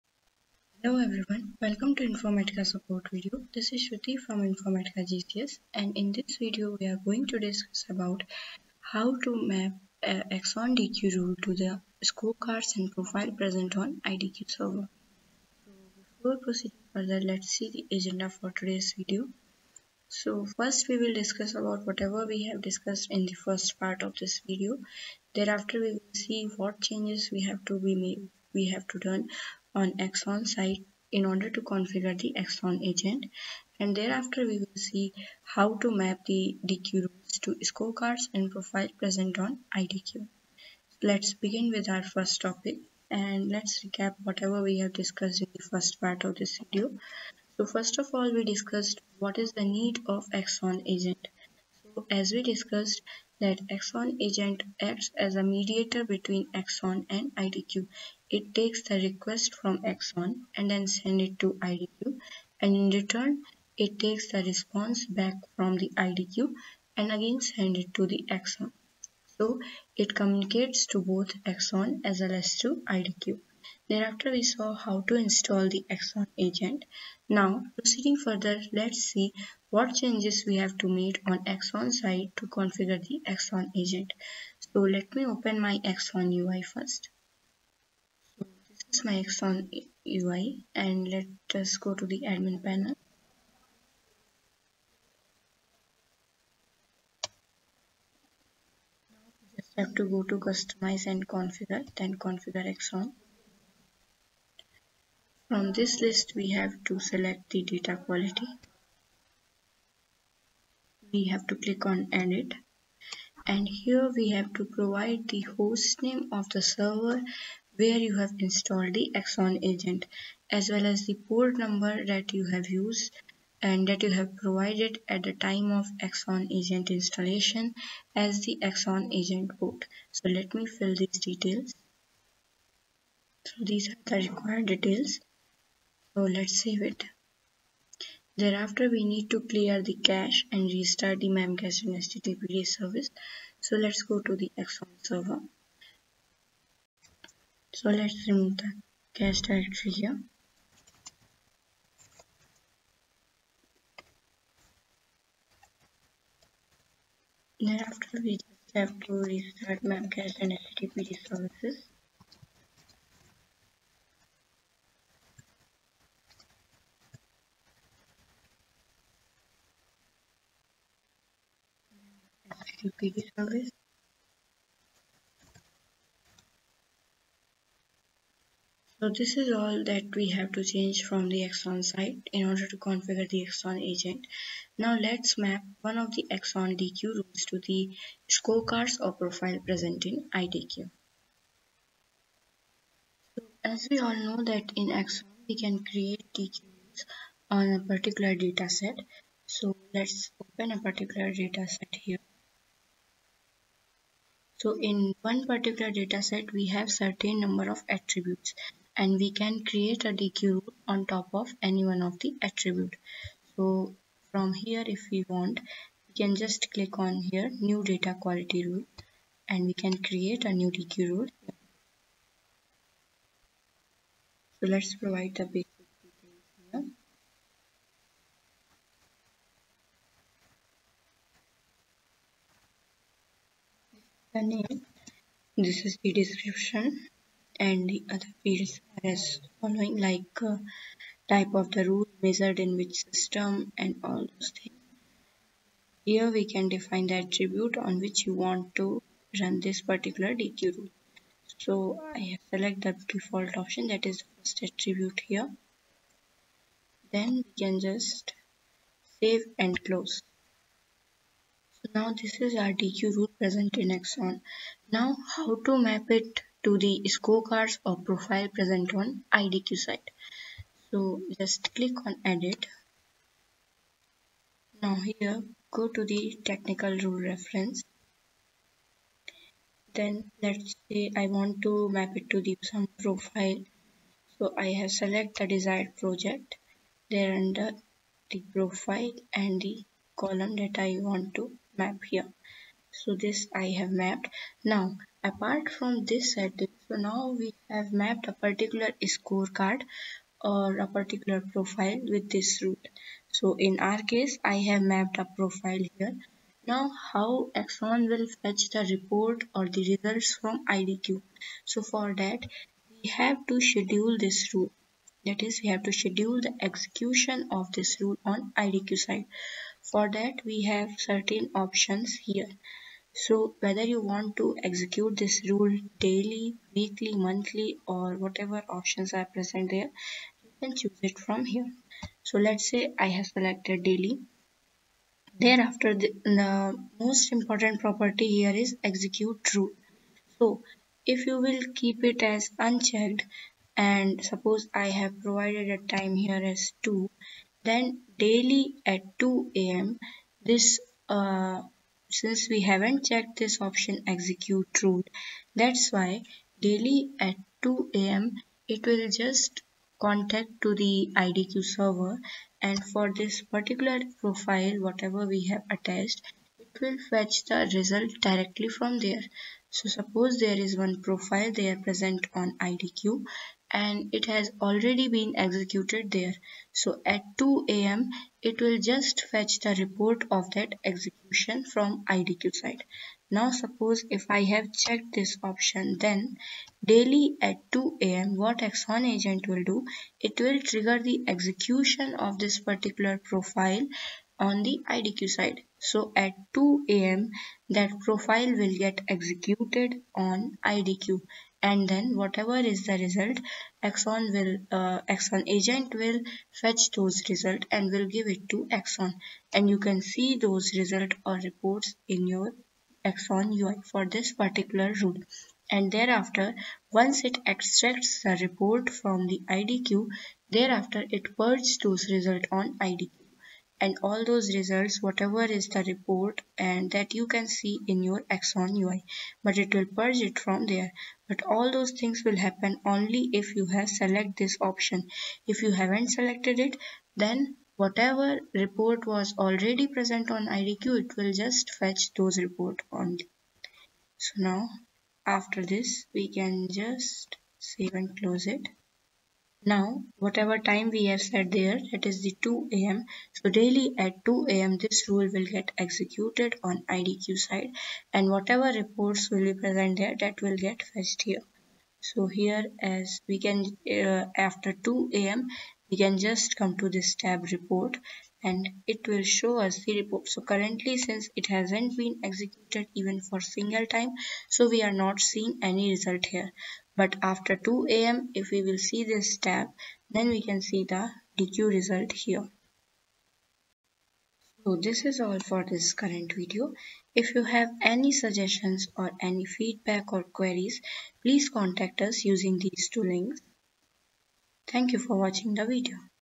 How to: Map Axon DQ Rules with IDQ scorecards/profiles
This video explains how to map Axon DQ Rules with IDQ scorecards/profiles.